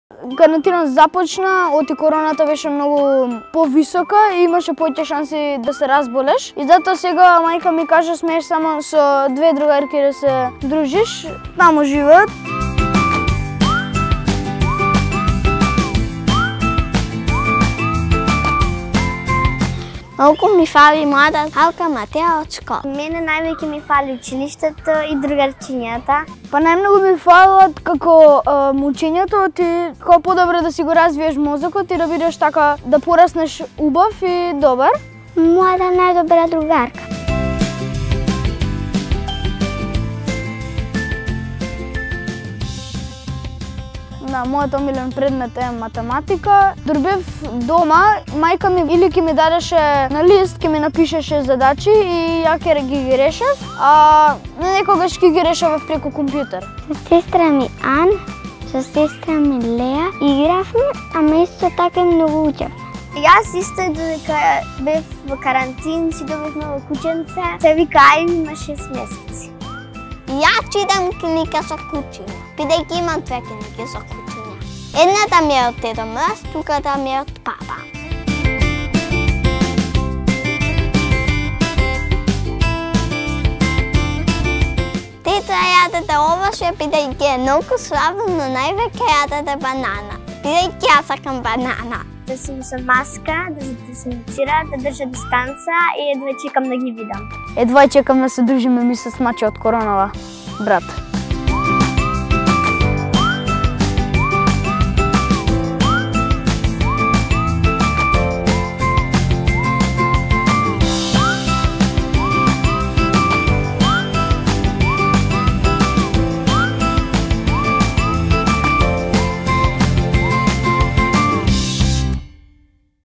Marko, Bora, Ana and Kaja
This year the school year started differently, and children must stay safe and continue learning.

Marko, Bora, Ana and Kaja have some useful tips on how to protect yourself from coronavirus. Check it out!